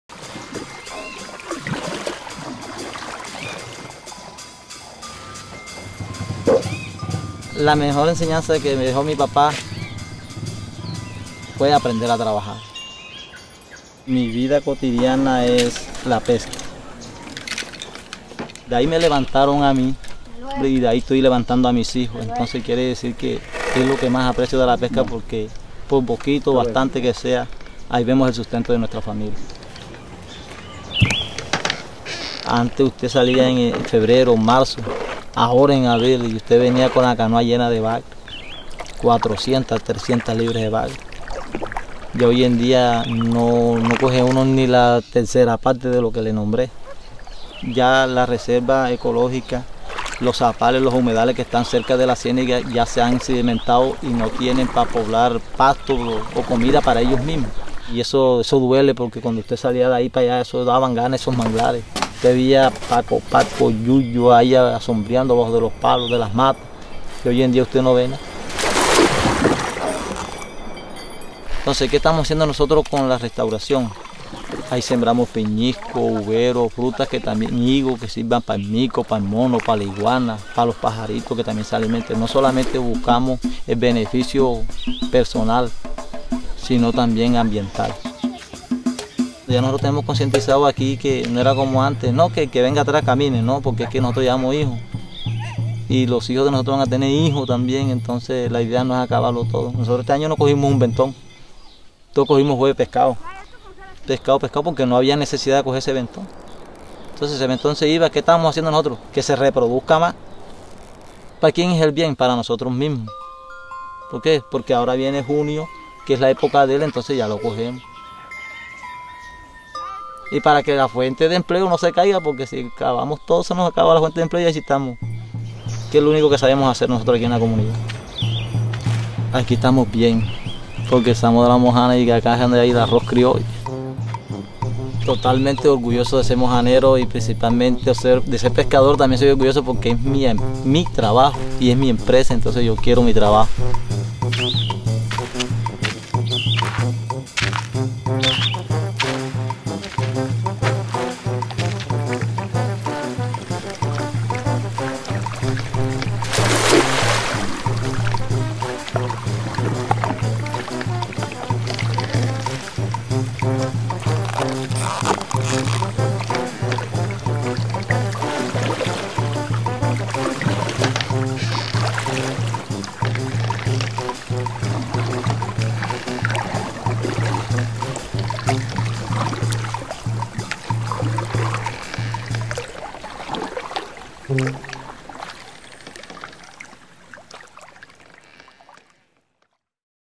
Pesca. La vida en un territorio anfibio.
Por siglos, campesinos de La Mojana han convivido con los ecosistemas de humedal, realizando actividades de pesca, agricultura de pancoger, caza de subsistencia o ganadería trashumante, cuyas técnicas las han aprendido de generación en generación. Los mojaneros usan ciénagas, caños, zapales y ríos como medio de transporte, recreación y fuente de materias primas para la construcción de viviendas y la elaboración de utensilios de cocina, herramientas o artesanías. Esto es orgullo mojanero.

Los mojaneros son herederos de los zenú, cultura anfibia por excelencia, y de conocimientos que les ha permitido adaptarse a su territorio y a los ciclos naturales de sequía e inundación que suceden durante el año. Por eso se reconocen como “hombres de agua” con un fuerte arraigo por su territorio anfibio, a pesar de adversidades como las intensas inundaciones de 2010 y los efectos de cambio climático.

En La Mojana, adelantamos estrategias de restauración de humedales y recuperación de los modos de vida anfibios mojaneros, dirigidas a aumentar el bienestar de las comunidades locales y a reducir el riesgo y la vulnerabilidad de las mismas frente al cambio climático.